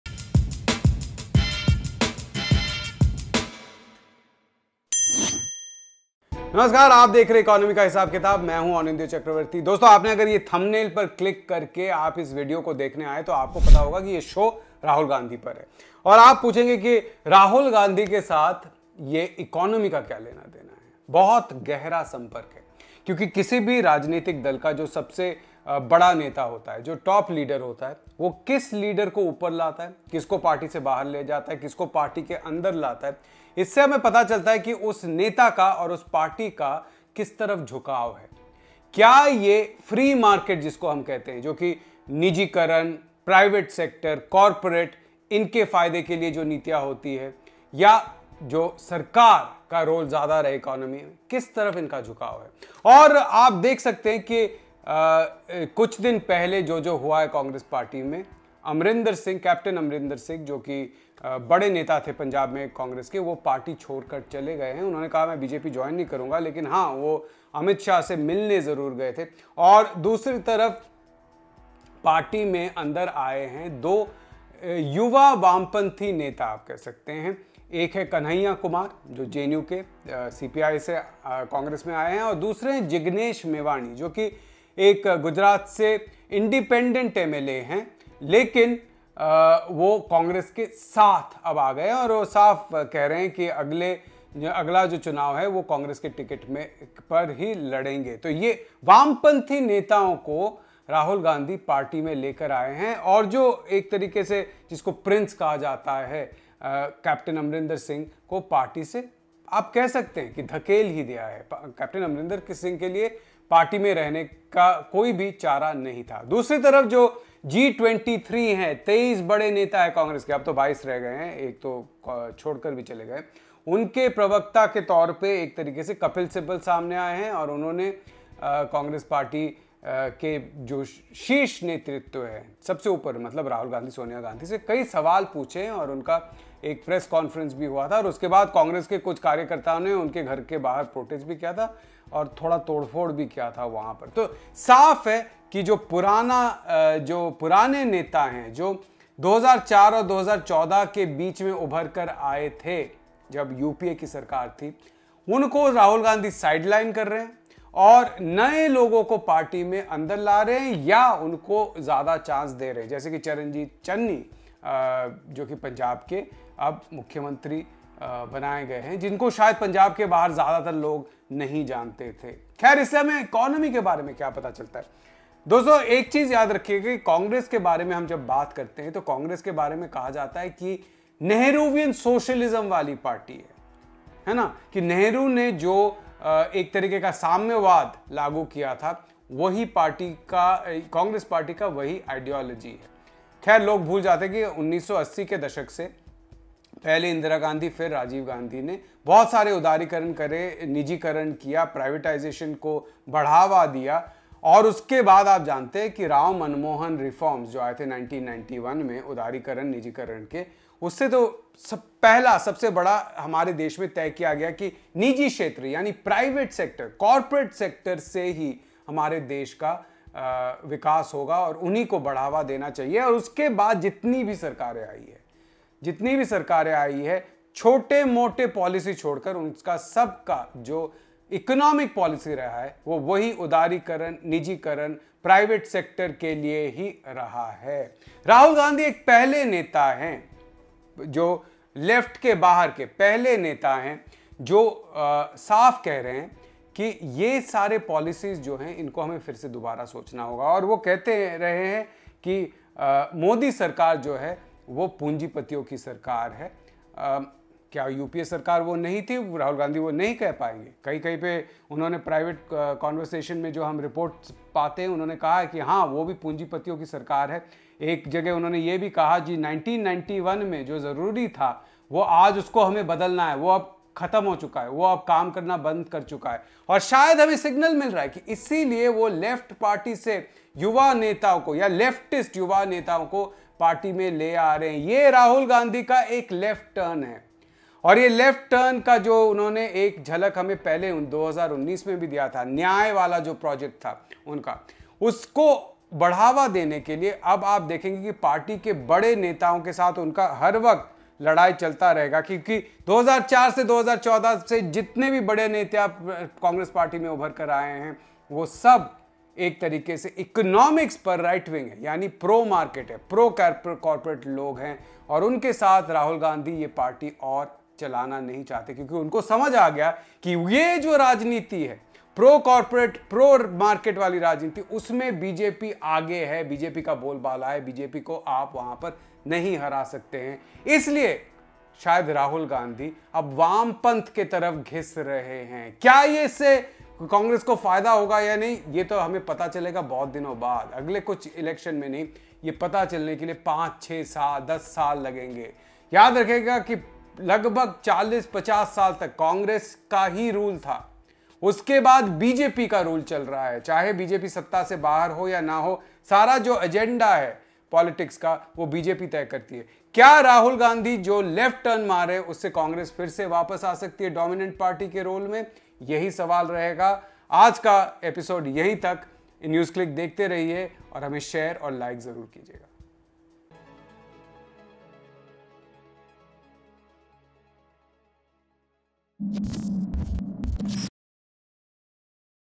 कांग्रेस में बड़ा भूकंप
राहुल गाँधी ने एक तरफ अमरिंदर सिंह को पार्टी से बाहर कर दिया तो दूसरी तरफ वामपंथी युवा को पार्टी से जोड़ा। इससे पार्टी के अंदर की लड़ाई खुलकर सामने आ गयी है.  क्या राहुल पार्टी को वामपंथ की तरफ ले जा रहे हैं? जवाब ढूंढ रहे हैं ऑनिंद्यो